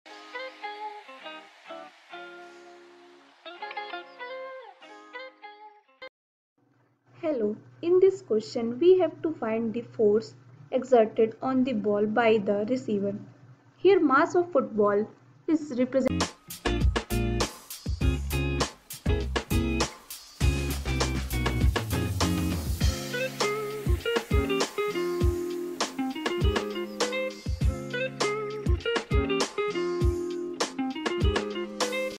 A 0.46 kg football is thrown with a velocity of 18 m/s to the right. A stationary receiver catches …
A 0.46 kg football is thrown with a velocity of 18 m/s to the right. A stationary receiver catches the ball and brings it to rest in 0.030 s. What is the force exerted on the ball by the receiver?